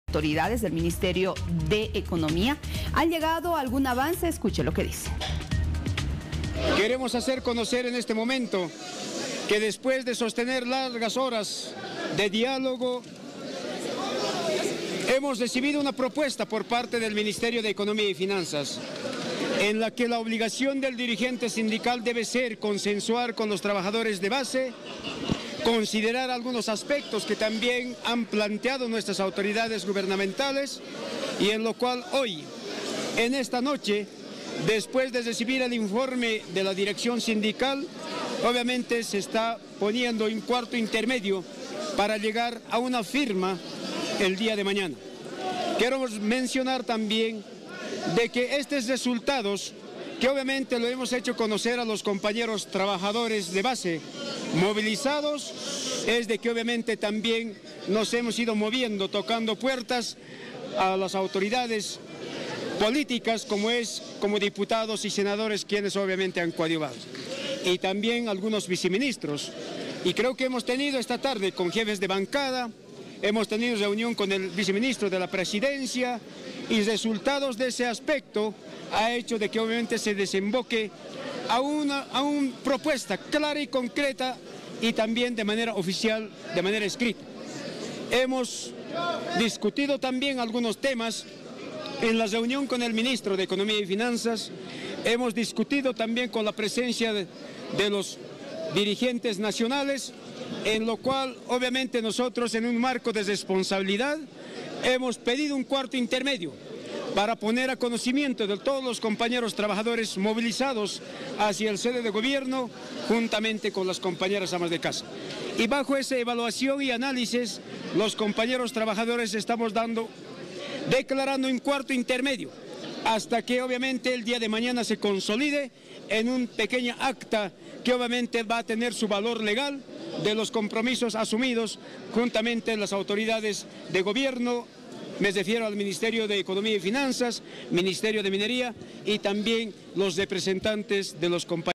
Mineros se encuentran en cuarto intermedio
Mineros informarán las propuestas planteadas por el Gobierno.
Luego de largas mesas de diálogo  los mineros del Colquiri señalaron que recibieron una propuesta del Ministerio de Economía.